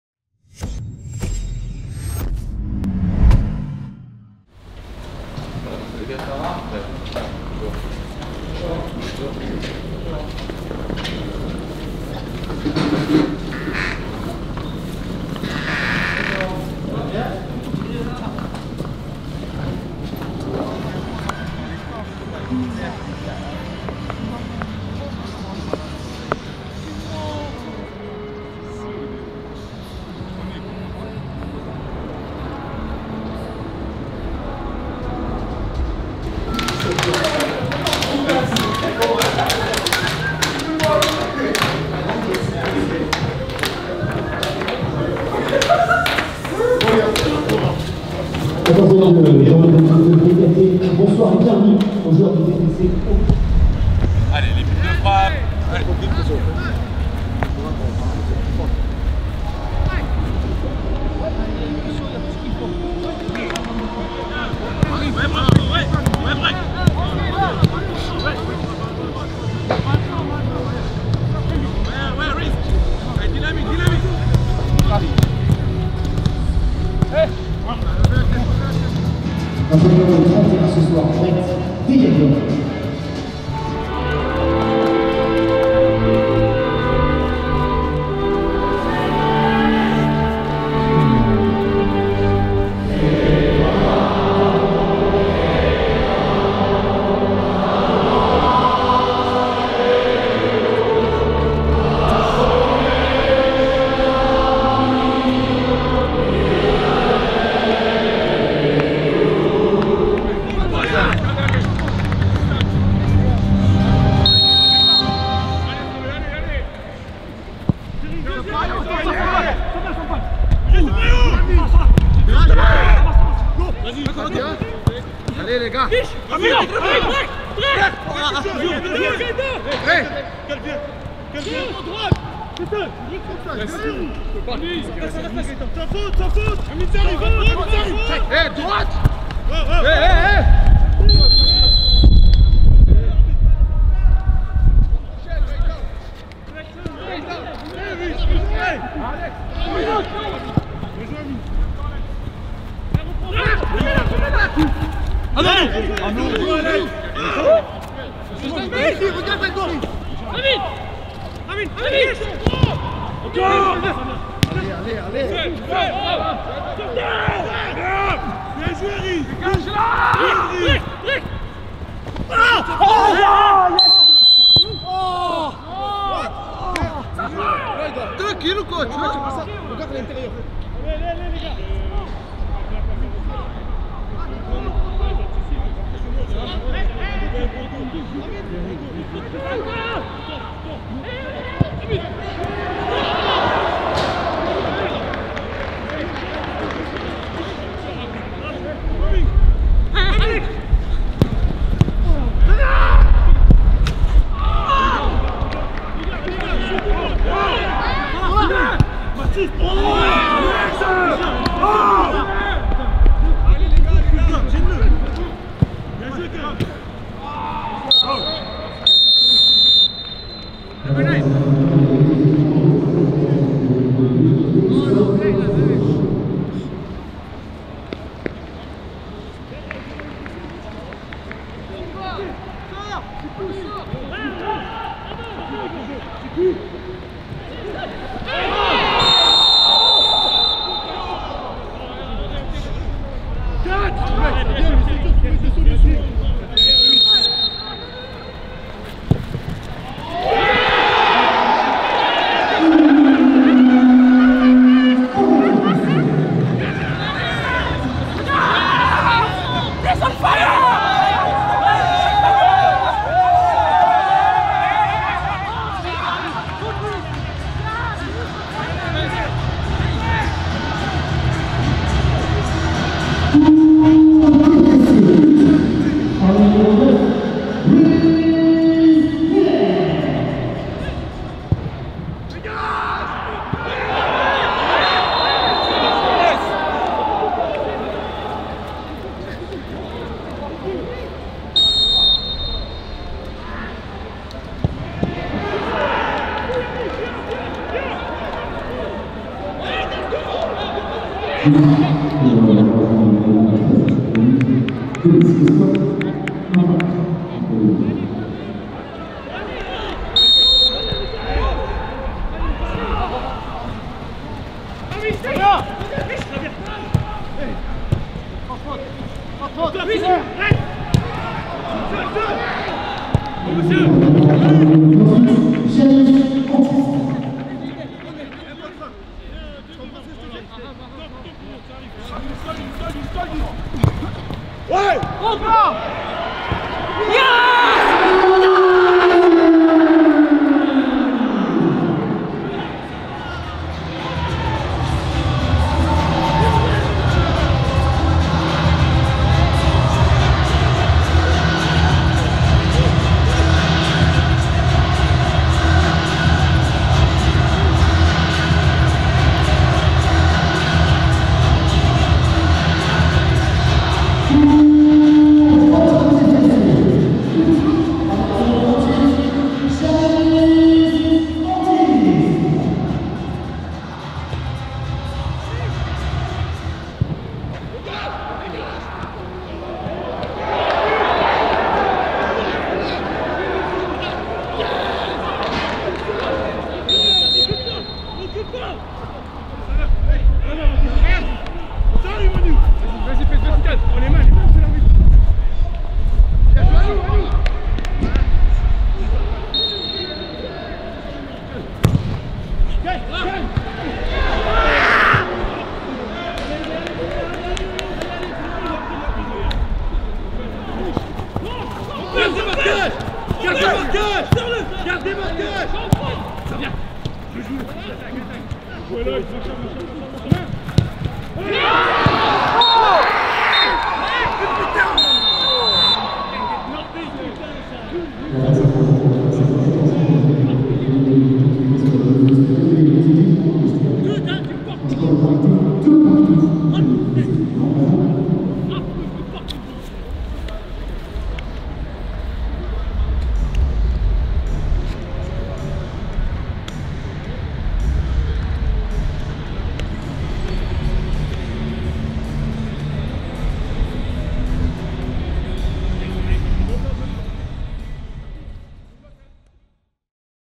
TFCEAG TFC-Guingamp au plus près du terrain
Comme après chaque rencontre au Stadium, notre caméra vous propose de revivre les aventures des Violets avec des images et un angle inédits. Et pour ce match face à Guingamp, comptant pour la 14ème journée de @ligue2fr (2-2), nous avons suivi le milieu de terrain belge Brecht Dejaegere, sans rater pour autant le coup-franc de Rhys Healey ou le but opportuniste de Janis Antiste.

Superstitieux? Pied droit ou pied gauche, faites votre entrée sur la pelouse!